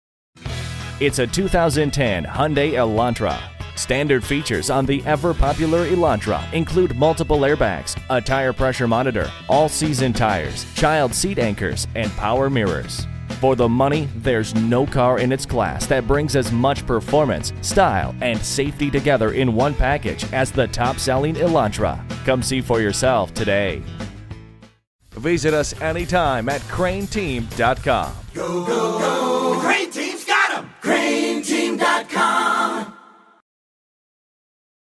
2010 Hyundai Elantra Little Rock AR Bryant, AR 3HS9713A - SOLD
Year: 2010
Make: Hyundai
Model: Elantra
Trim: GLS
Engine: Gas I4
Transmission: Automatic
Color: BLACK
Mileage: 29555
Stock #: 3HS9713A
a onclick="cancelEvent(event);ceP(this);" href="javascript:;"Get Internet Price/aTHIS ELANTRA IS A AUOT-MATIC WITH ALOT OF OPTIONS. GREAT ON GAS AND MORE..

Address:
11701 Colonel Glenn Rd